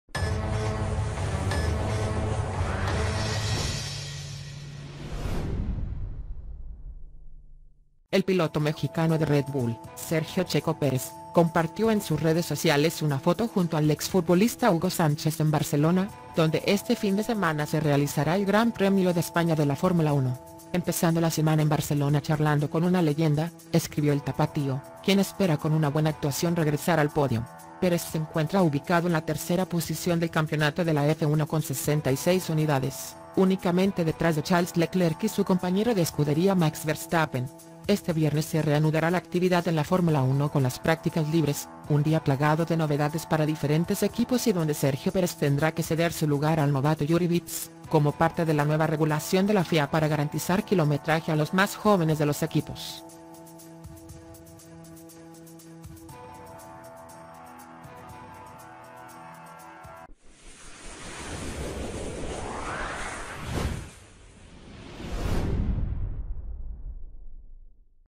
¡Leyendas! Checo Pérez presume encuentro con Hugo Sánchez previo al GP de España
El piloto mexicano de Red Bull, Sergio 'Checo' Pérez, compartió en sus redes sociales una foto junto al exfutbolista Hugo Sánchez en Barcelona, donde este fin de semana se realizará el Gran Premio de España de la Fórmula 1.

“¡Empezando la semana en Barcelona charlando con una leyenda! spanishgp”, escribió el tapatío, quien espera con una buen actuación regresar al pódium.

Pérez se encuentra ubicado en la tercera posición del campeonato de la F1 con 66 unidades, únicamente detrás de Charles Leclerc y su compañero de escudería Max Verstappen.

Este viernes se reanudará la actividad en la Fórmula 1 con las prácticas libres, un día plagado de novedades para diferentes equipos y donde Sergio Pérez tendrá que ceder su lugar al novato Juri Vips, como parte de la nueva regulación de la Federación Internacional de Automovilismo (FIA) para garantizar kilometraje a los más jóvenes de los equipos.